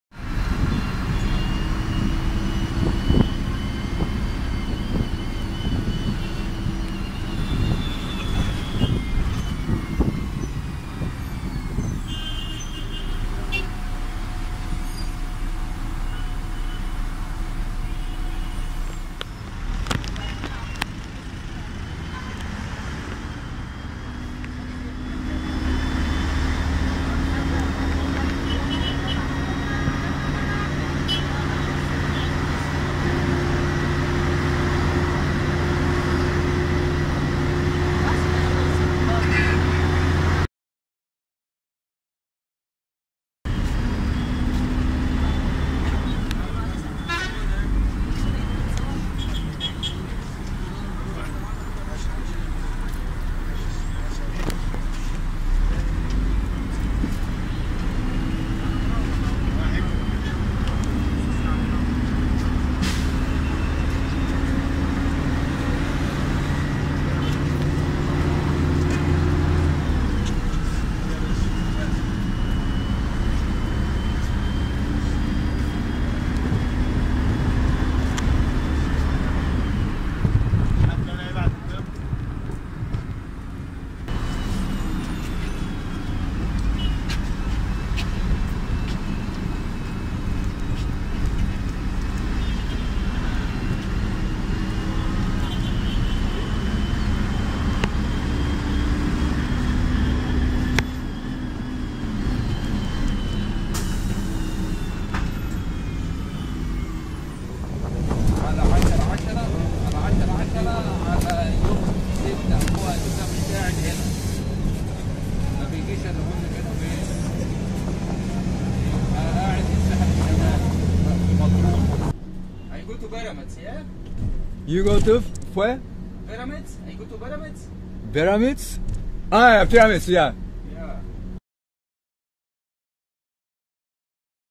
世界のバス窓から エジプト カイロ市内の風景
市バスの中から撮った、カイロのストリート風景です